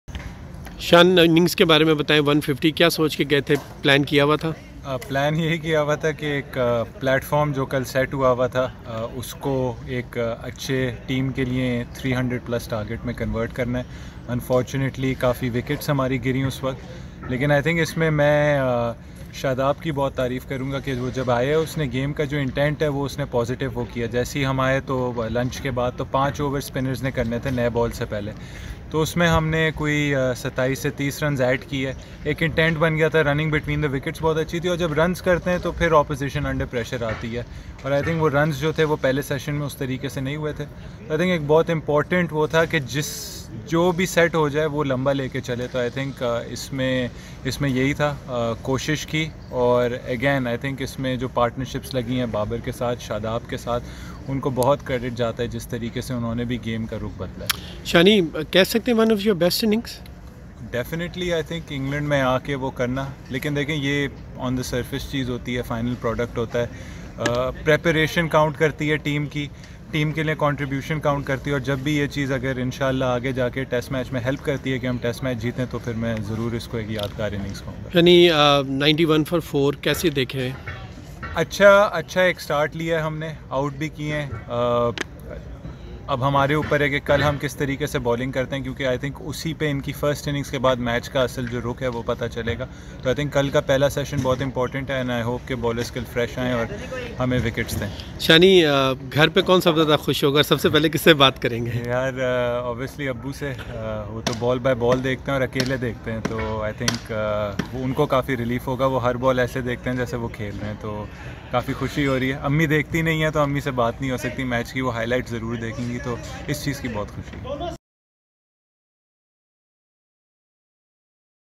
Pak vs Eng: Shan Masood says third day of the match would be very crucial - BBC URDU
پاکستان بمقابلہ انگلینڈ ٹیسٹ میچ کے دوسرے دن کے احتتام پر ایک انٹرویو میں پاکستانی کھلاڑی شان مسعود کا کہنا تھا کہ اگر پاکستان یہ میچ جیت گیا تو یہ ان کی یادگار اننگ ہو گی۔ ان کے مطابق تیسرے دن کا پہلا سیشن بہت اہم ہے اور وہ امید کرتے ہیں کہ باوٴلرز وکٹیں لیں گے۔
اگر وہ میچ جیت گئے تو ان کا کہنا ہے ’سب سے پہلے ابو سے بات کرنا پسند کروں گا، وہ میچ بال بائے بال دیکھتے ہیں امی سے [اس بارے میں] بات ہو نہیں سکتی کیونکہ وہ میچ نہیں دیکھتیں‘
انھوں نے مزید کیا کہا، فرقان الٰہی کی اس ویڈیو میں دیکھیں